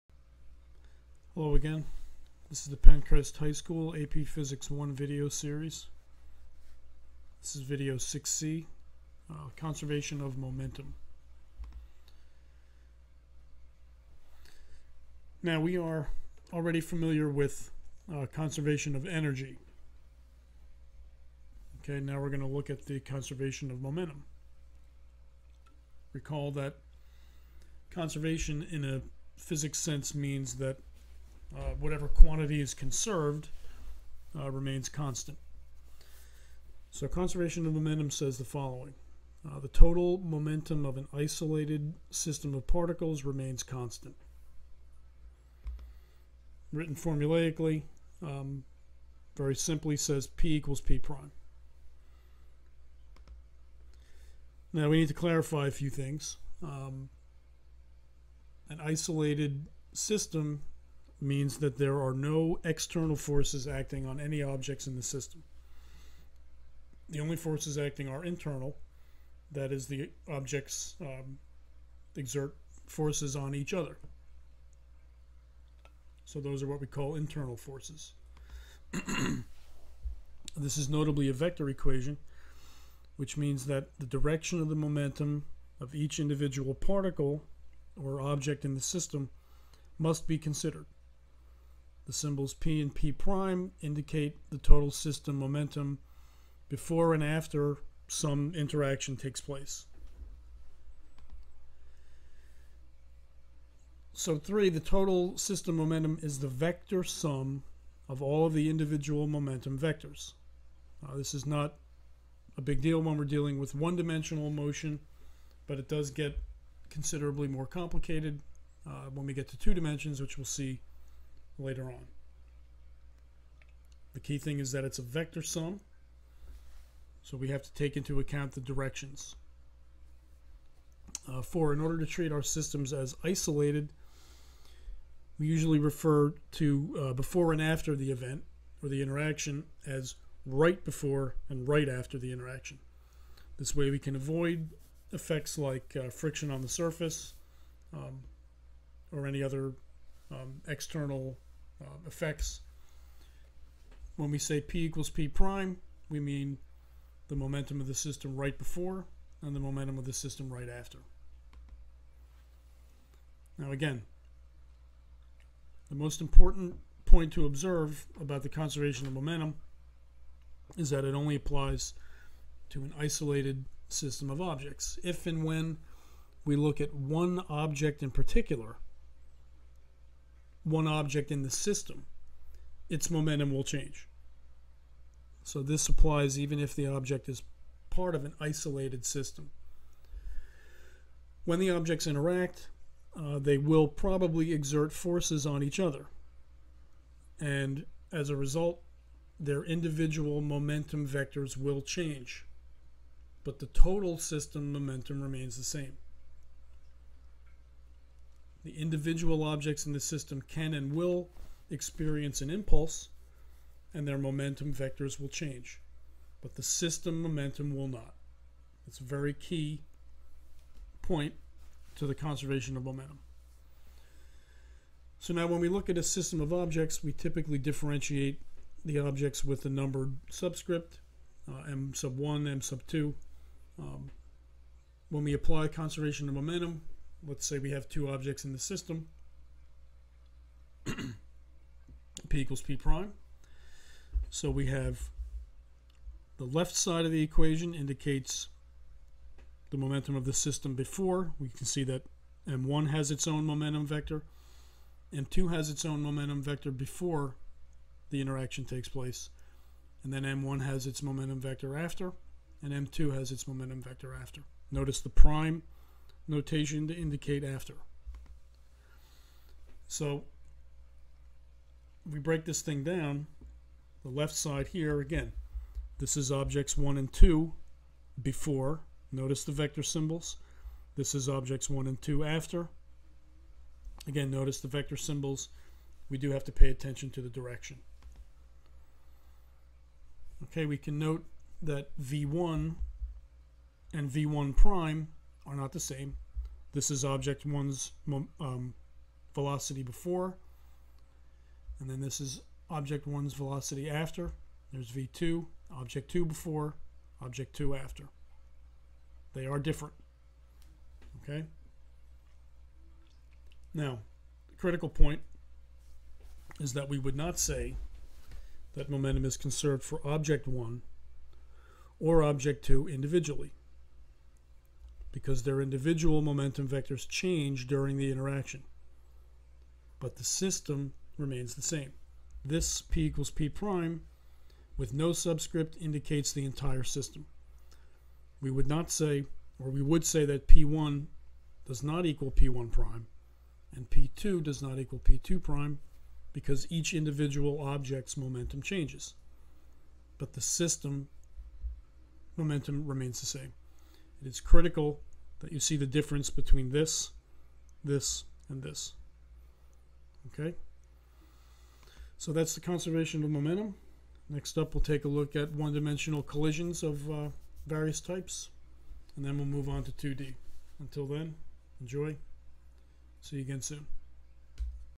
AP Physics 1 6C Momentum Conservation
Conservation of momentum, its vector nature, isolated systems